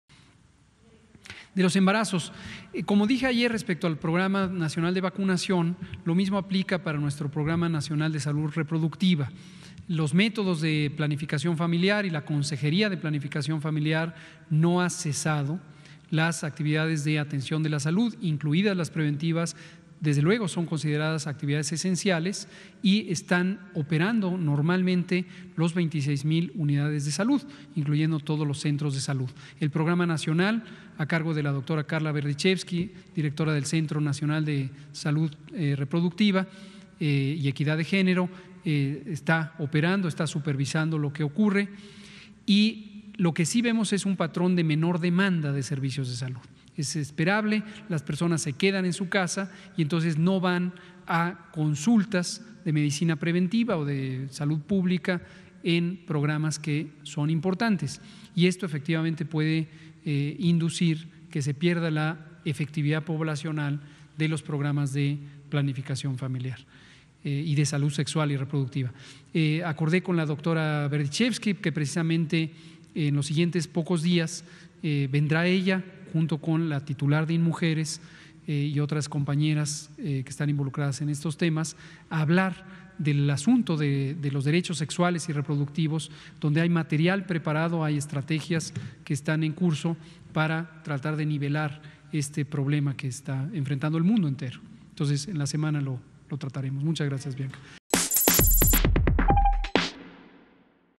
Es considerado actividad esencial el Programa Nacional de Salud Reproductiva: López-Gatell
El Programa Nacional de Salud Reproductiva no ha cesado las actividades de atención a la salud, estas son consideradas actividades esenciales y están operando normalmente las 26 mil unidades de salud, aseguró Hugo López-Gatell, subsecretario de Prevención y Promoción de la Salud.